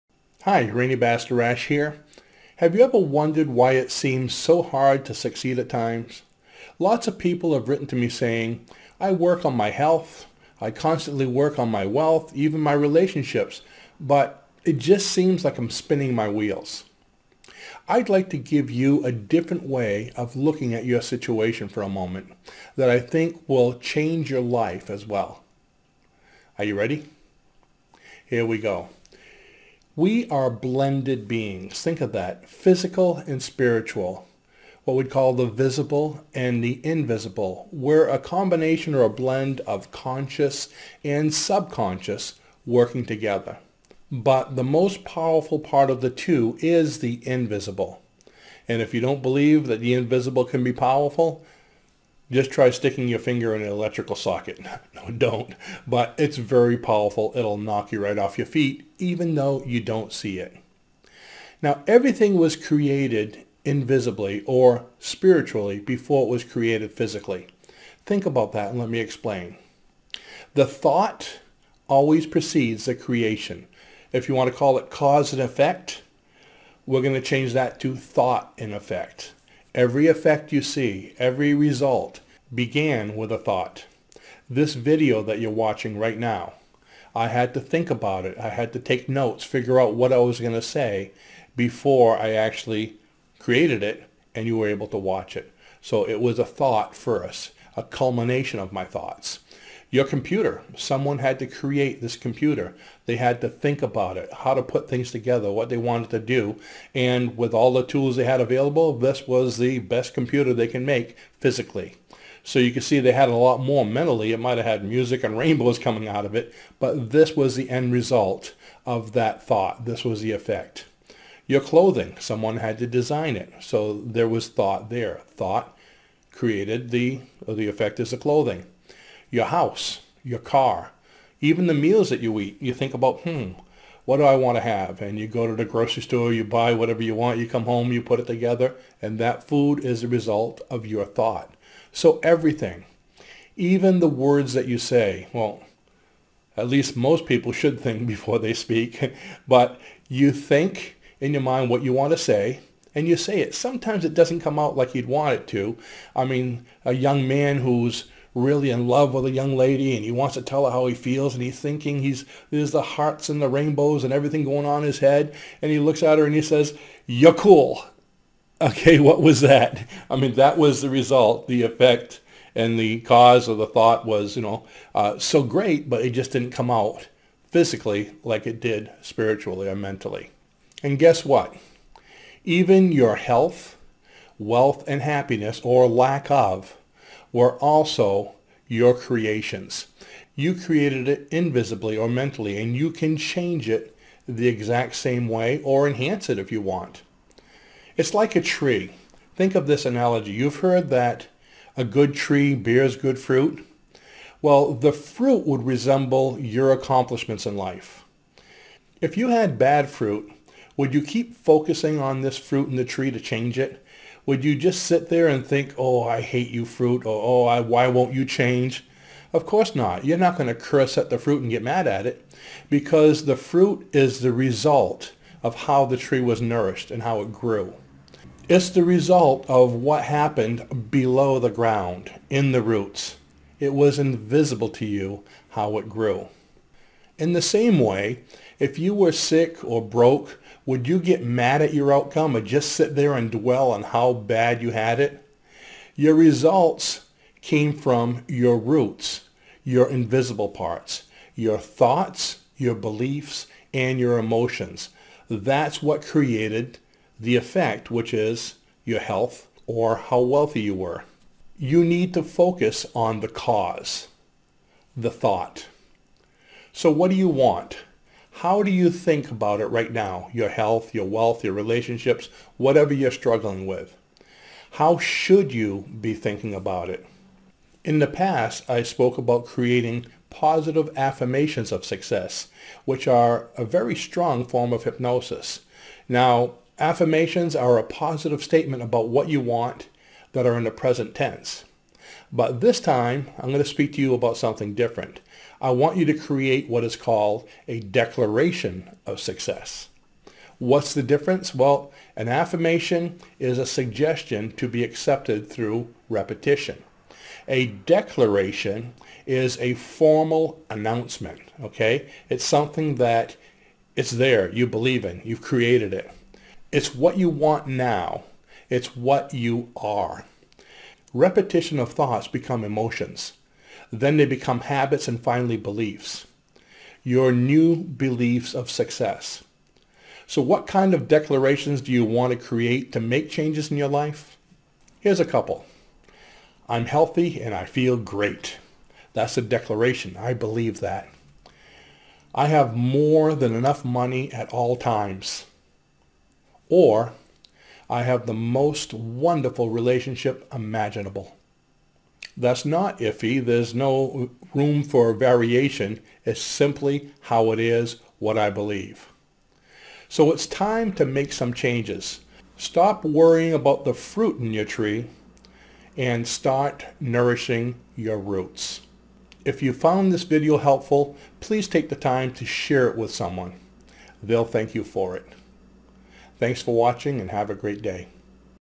Success Mindset for Health, Wealth and Happiness
Have you ever wondered why it seems so hard to succeed at times? Here's a different way of looking at your situation that will change the way you look at your life as well. Thought and Effect